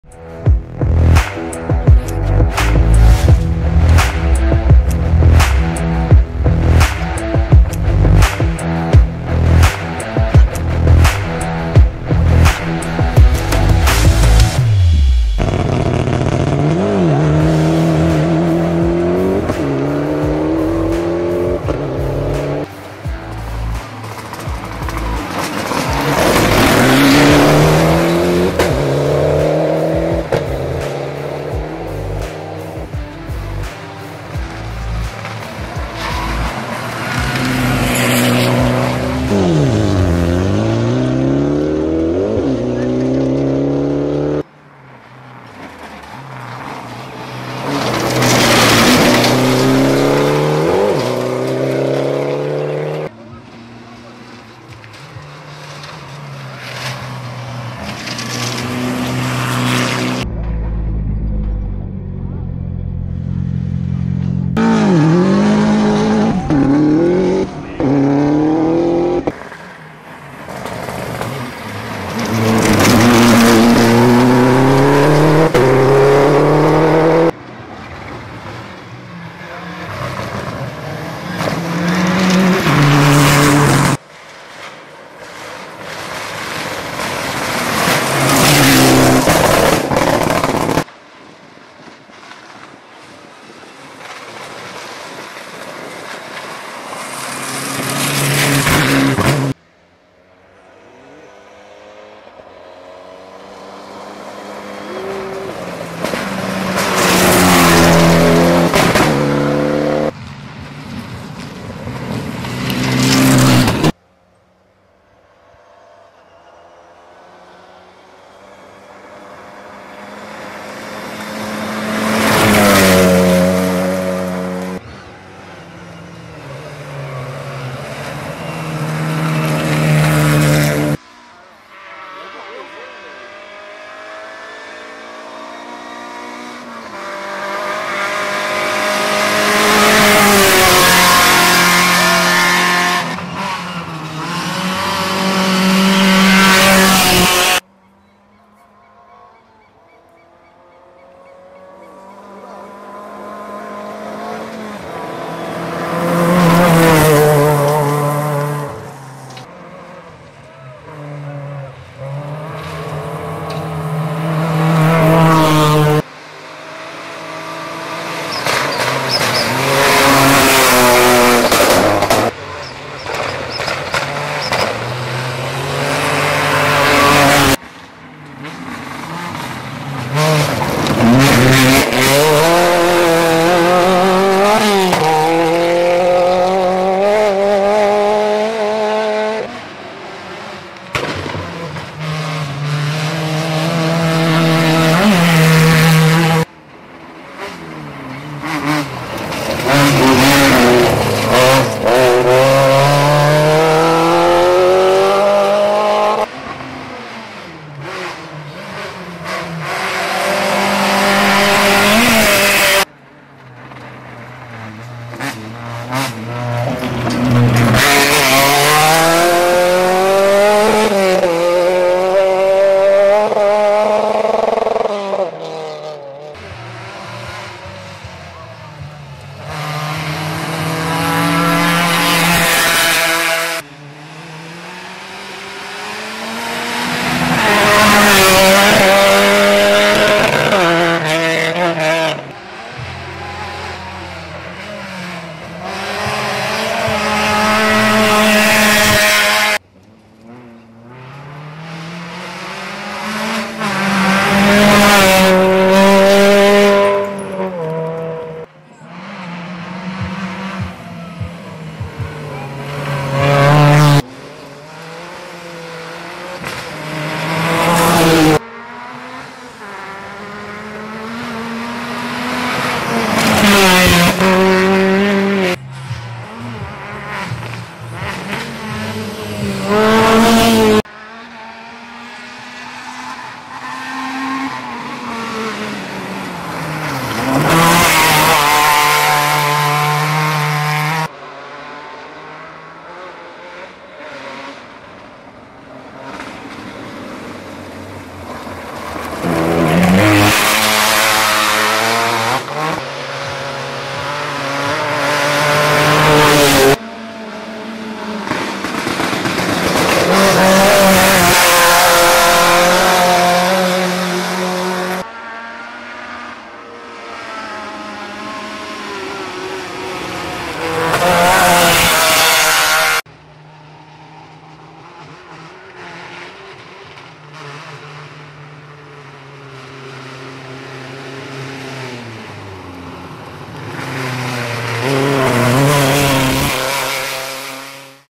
SS Pinheirinho 1 e 2 - Rally Rio Negrinho 2023
Imagens das 2 primeiras passagens na SS Pinheirinho, de 7,80 Km, disputada neste sábado (23/09) no 5º evento do Brasileiro de Rally de Velocidade em Rio Negrinho (SC).

Saiba mais em rallybr.com.br e na cobertura @rallybroficial do Instagram e do canal Whatsapp